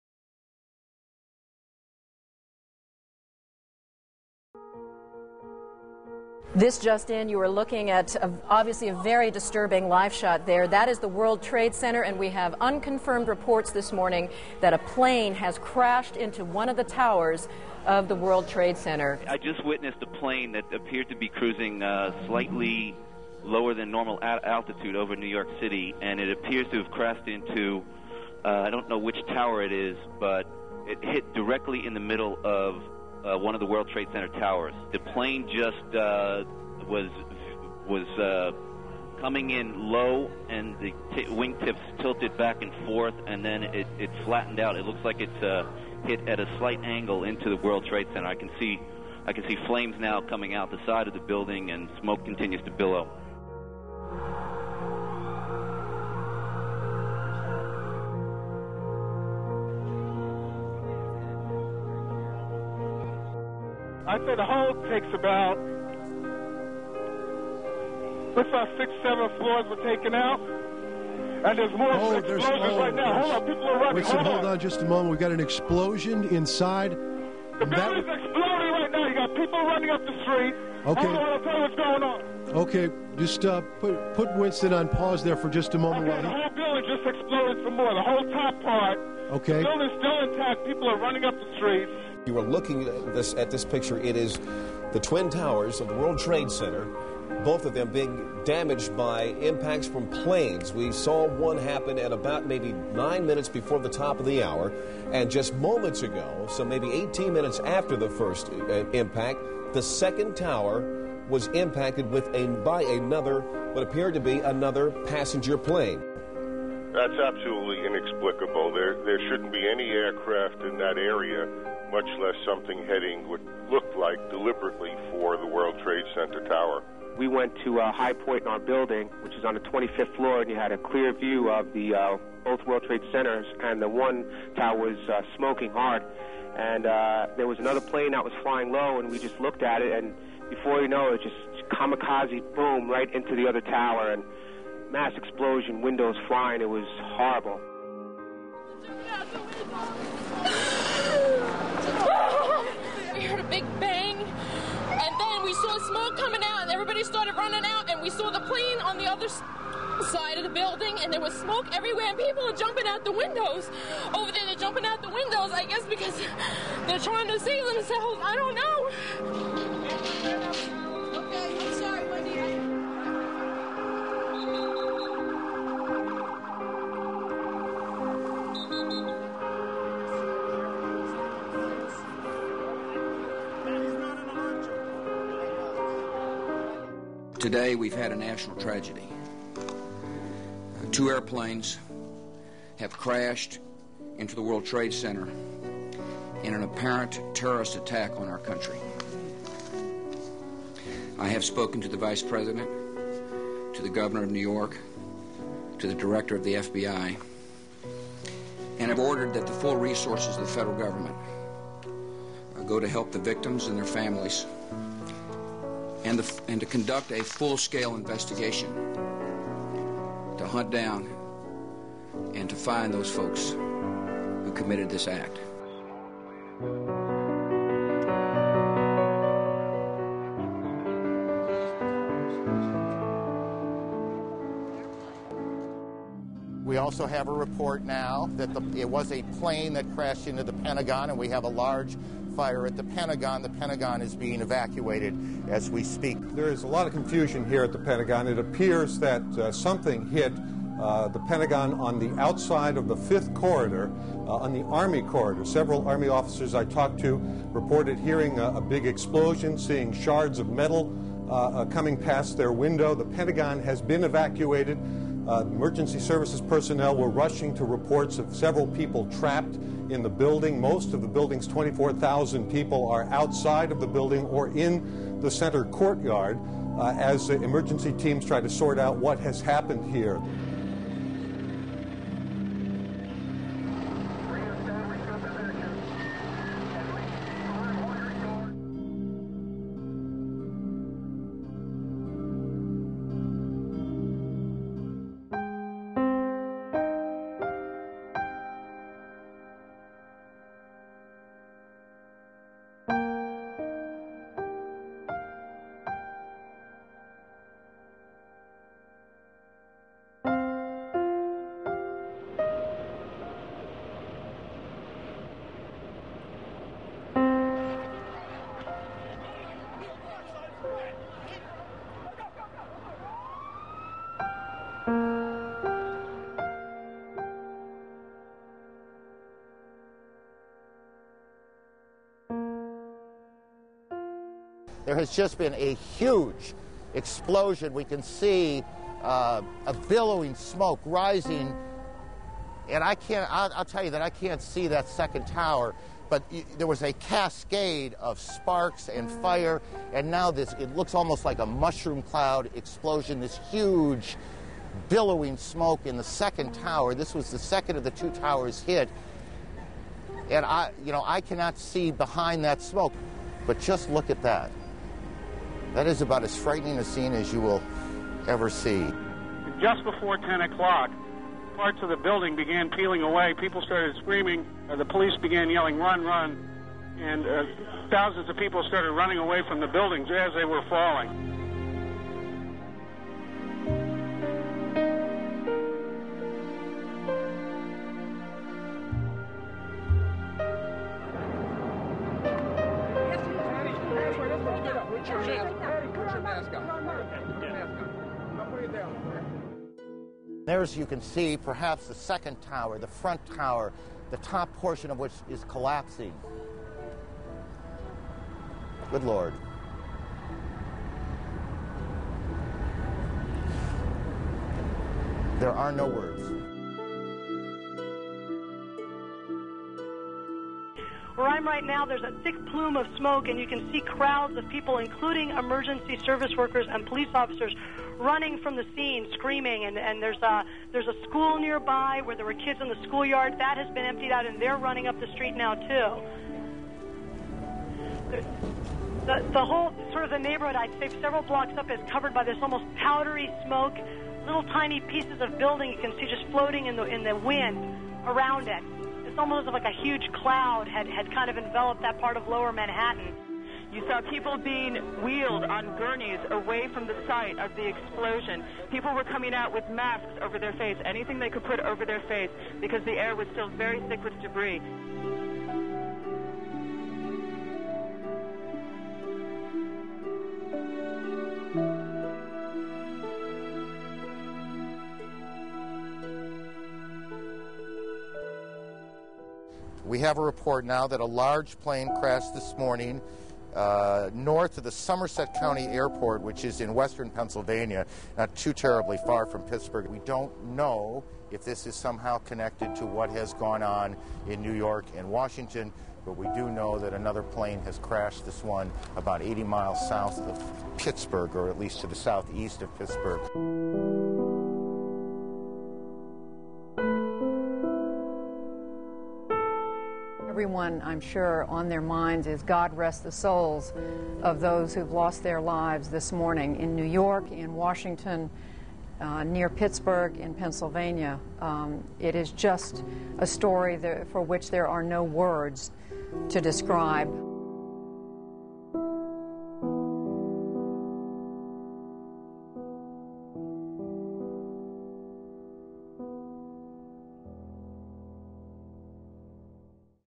Look back at how September 11 unfolded
On the eleventh anniversary of the September 11 terrorist attacks, CNN looks back at the events as they unfolded that day.